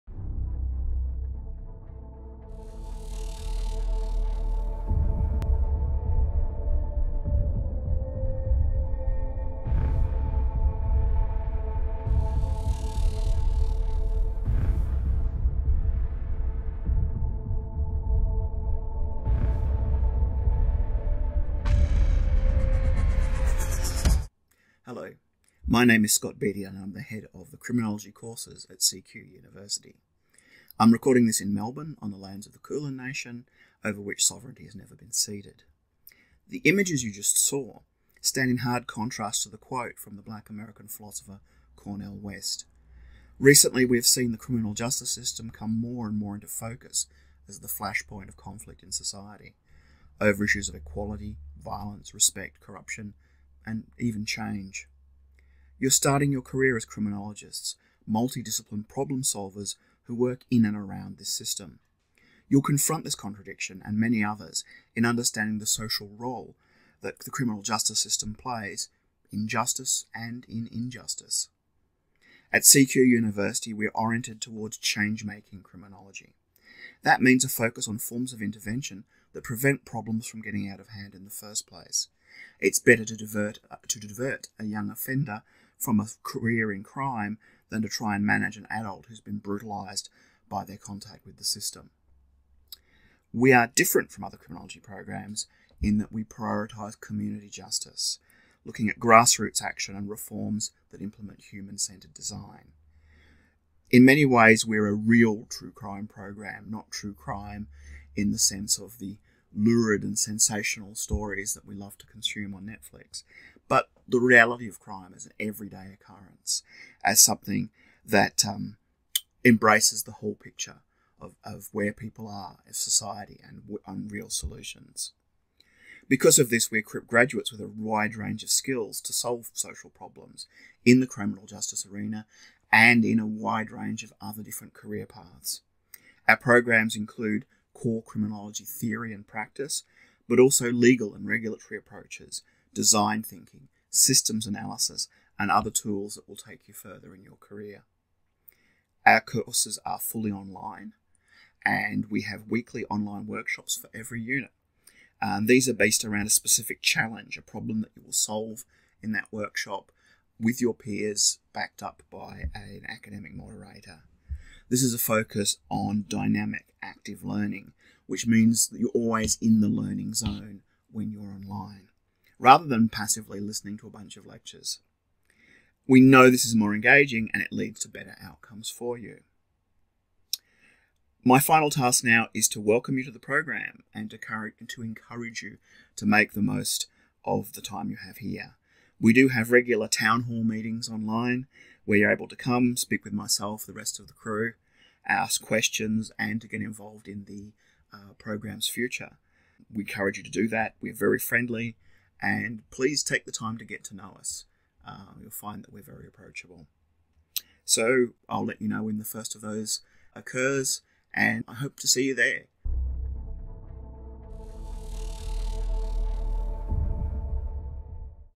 Welcome to Criminology at CQUniversity
Presented by Head of Course, Scott Beattie